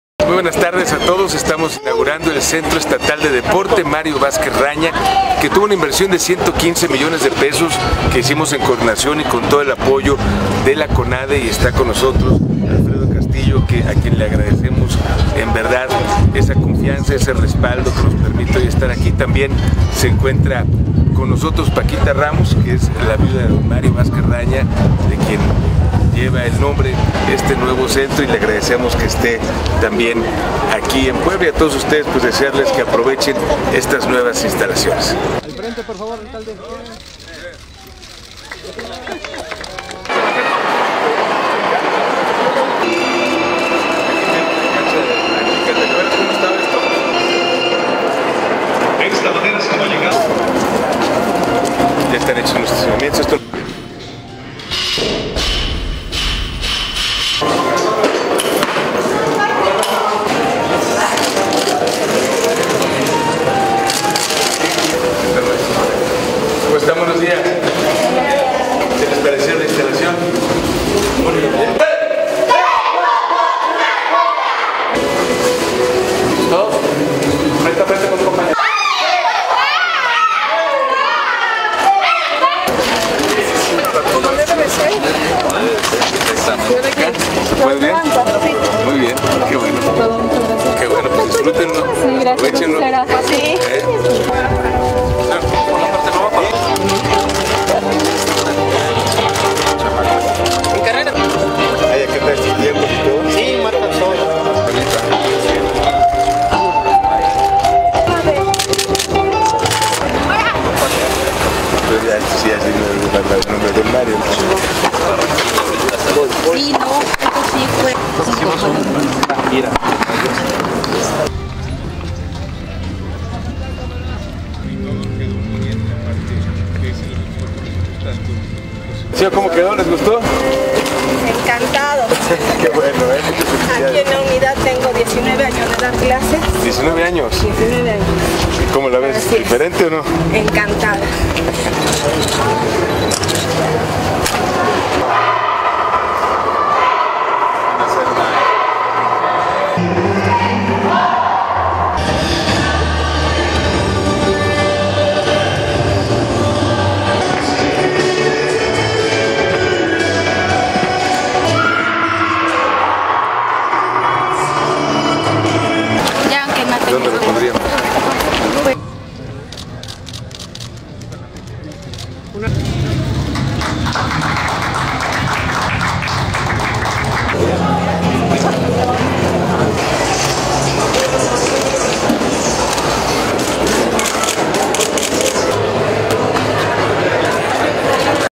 Moreno Valle inagurua el Centro Estatal del Deporte
¿Te gusta hacer ejercicio? Ahora, en el nuevo Centro Estatal del Deporte los poblanos y los atletas de alto rendimiento podrán preparase y disfrutar de una de las mejores instalaciones de México.

Agradezco el apoyo de la CONADE y la presencia de Paquita Ramos viuda de Mario Vázquez Raña, un gran promotor del deporte en México.